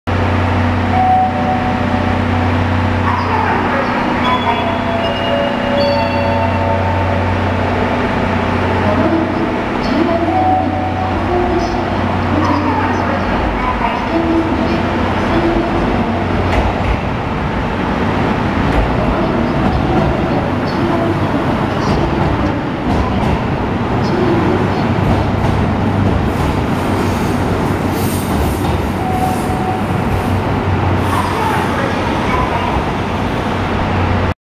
札沼線普通札幌行 札幌駅到着
終点の札幌に到着するキハ40+キハ141系。